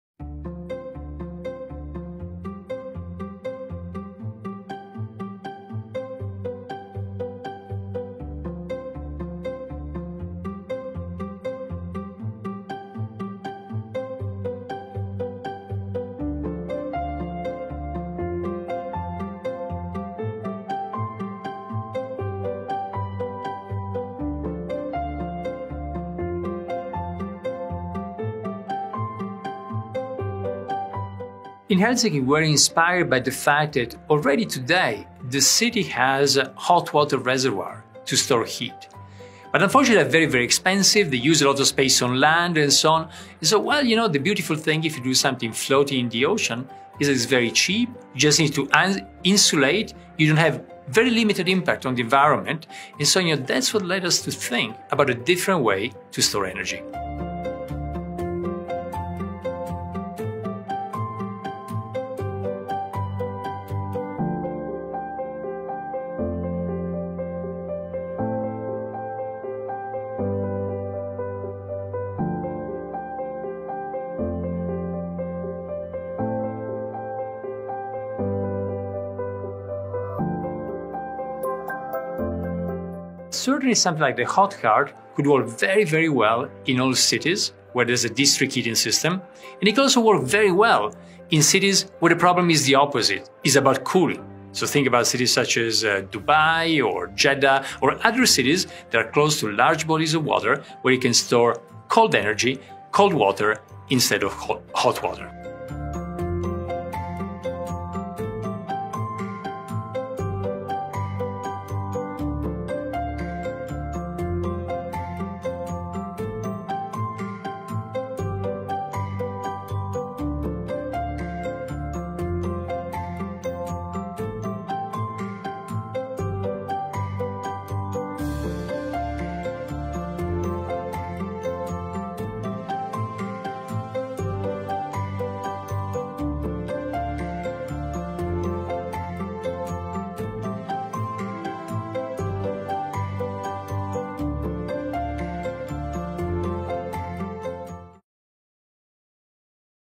Helsinki is building artificial islands to heat the city renewably.store energy as hot water
The floating reservoirs store energy as hot water until it’s needed to heat the city in winter.
Hot Heart has been selected by the Municipality of Helsinki as the winner of the Helsinki Energy Challenge, in recognition of its potential to de-carbonize the city’s district heating system before 2030. The project consists of a set of 10 cylindrical basins, each 225 meters in diameter. They collectively can hold up to 10 million cubic meters of water. Low- or negative-cost renewable energy is converted into thermally-heated water, stored inside and withdrawn into the city’s heat distribution channels during the winter.

When implemented, Hot Heart is expected to cover the entire heating needs of Helsinki, all without any carbon emissions and at an estimated cost 10% lower than today. Besides that, “floating forest” filled with the world’s key rainforest zones will also be planted on top of the basins, nurtured sustainably with the heat produced beneath them. They will raise the locals’ awareness towards the world’s ecosystems.